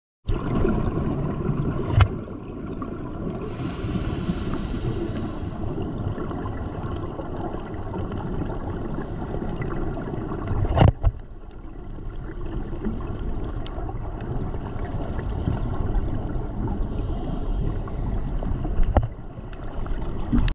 Eel in Turks and Caicos
Mad eel saying to stay away in Turks and Caicos